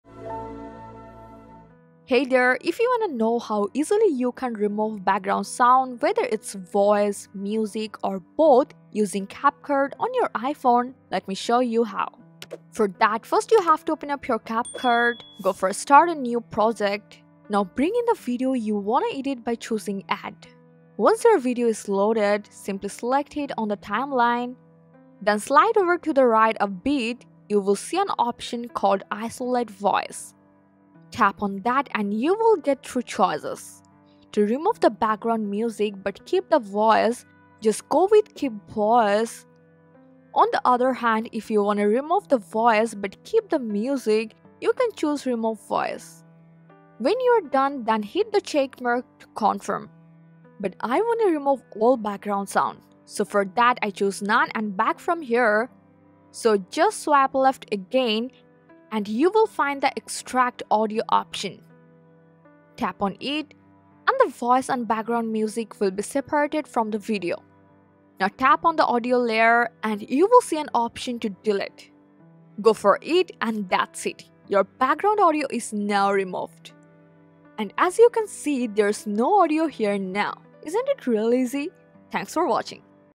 Remove Background Music In CapCut! [Keep Voice]
Struggling to remove the background Music from a video CapCut using any iPhone, including iPhone 16, 15, 14, 13, 12, and 11 Pro Max, but keep the voice or vice versa? Whether you want to delete the song, voiceover from a video, or clear all audio, including background noise, at once using the CapCut iOS App, this is the video for you.

In this quick guide, we will walk you through the step-by-step process to erase music, voice, or any sound from a video on CapCut using your iPhone. Here we’ll show you how to:

• Only remove song/ background music from a video, but keep the Voice using the CapCut App.
• Remove the Voice but keep the background Music on a video using CapCut on iPhone
• Clear the complete audio from a video using CapCut on iPhone.

0:00 Intro: Remove Background Music In CapCut
0:12 Remove only Voice or Music from the video
0:51 Remove all audio from CapCut